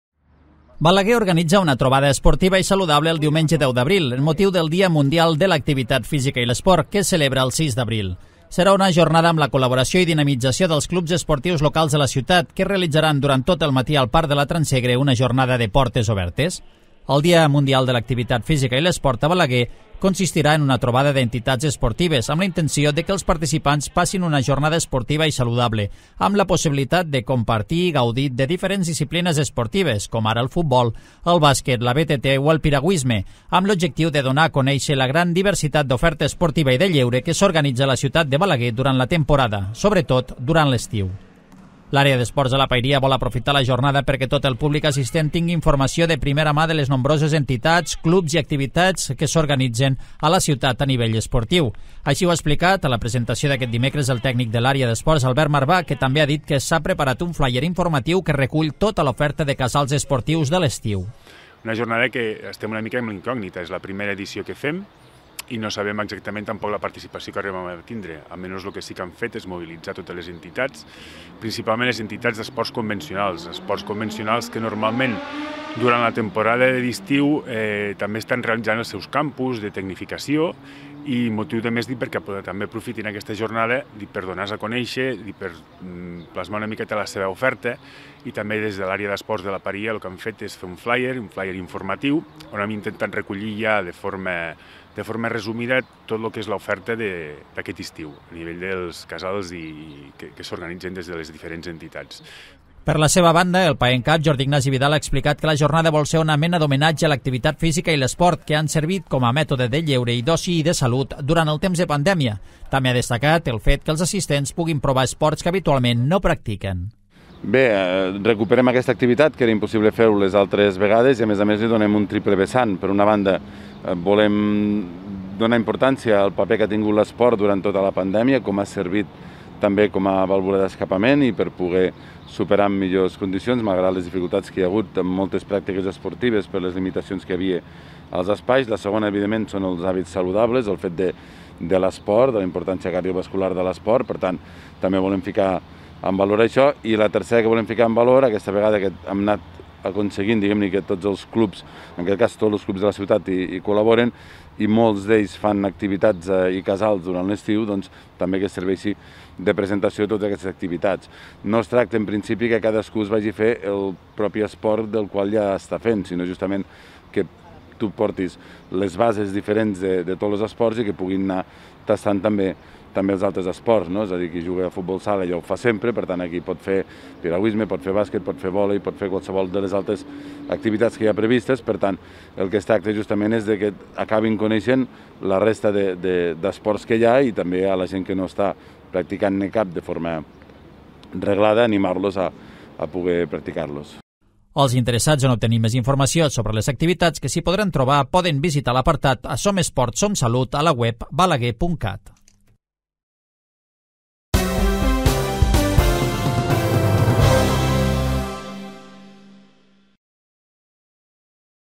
Presentació del Dia Mundial de l'Activitat Física i l'Esport a Balaguer
Balaguer celebrarà el Dia Mundial de l'Activitat Física i l'Esport el proper diumenge 10 d’abril amb una trobada esportiva i saludable. Serà una jornada de portes obertes que comptarà amb la col·laboració i dinamització dels clubs locals de la ciutat.